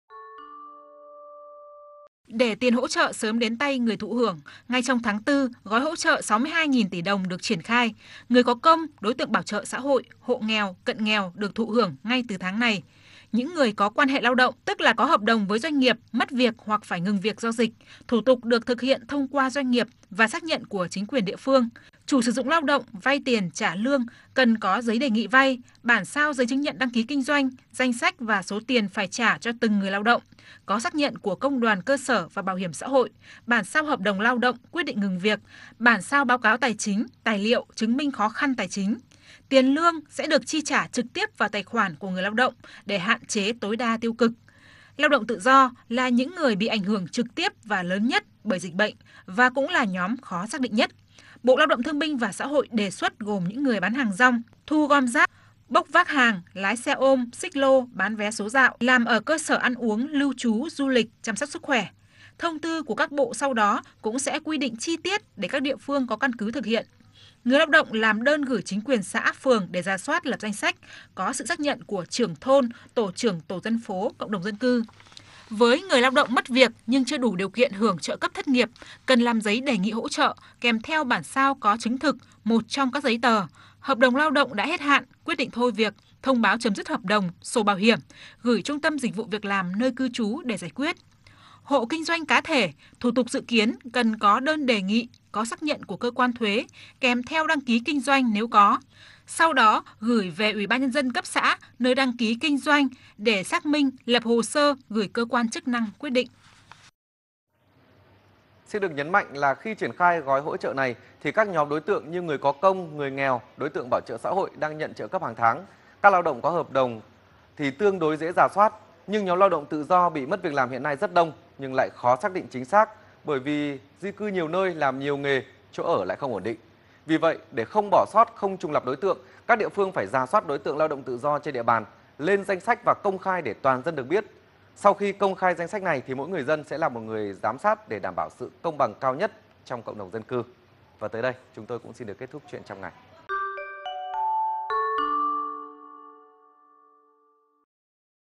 Ai sẽ được nhận tiền từ gói hỗ trợ 62.000 tỷ đồng? | VTC1
VTC1 | Số tiền Thủ tướng phê duyệt gói an sinh xã hội trị giá 62.000 tỷ đồng sẽ được đưa đến tay người dân trong tháng 4. Vậy, ai sẽ nhận được hỗ trợ và cần thủ tục, điều kiện gì để được nhận hỗ trợ?